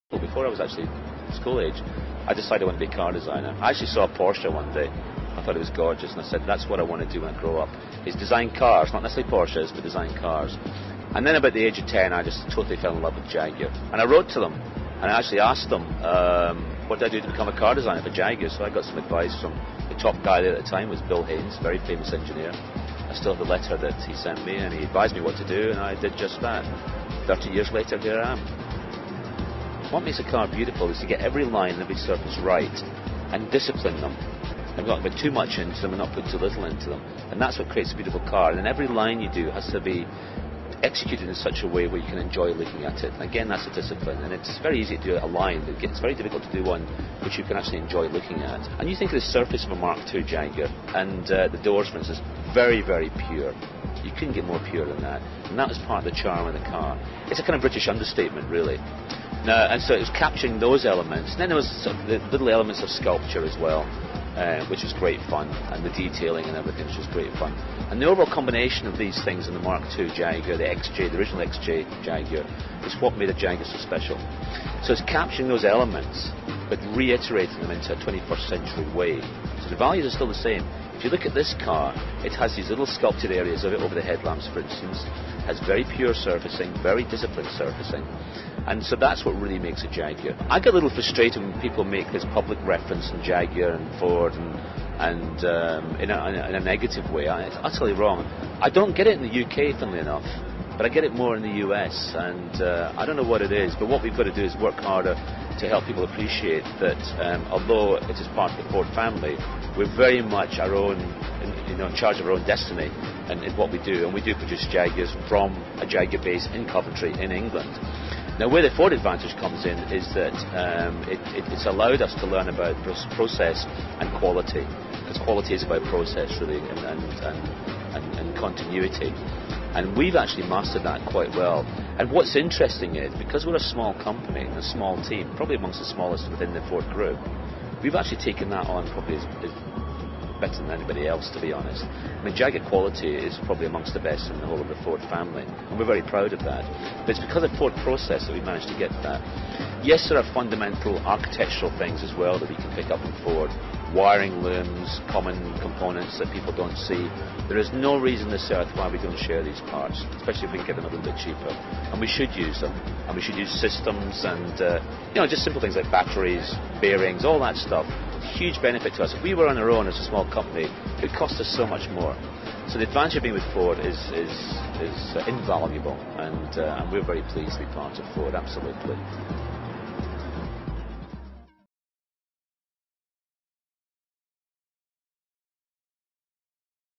Jaguar Designer Ian Callum Talks Auto Design
From Ford Auto Shows, Jaguar Designer Ian Callum talks shop about auto design, explaining how elements of sculpture and previous model designs go into new concepts like the C-XF.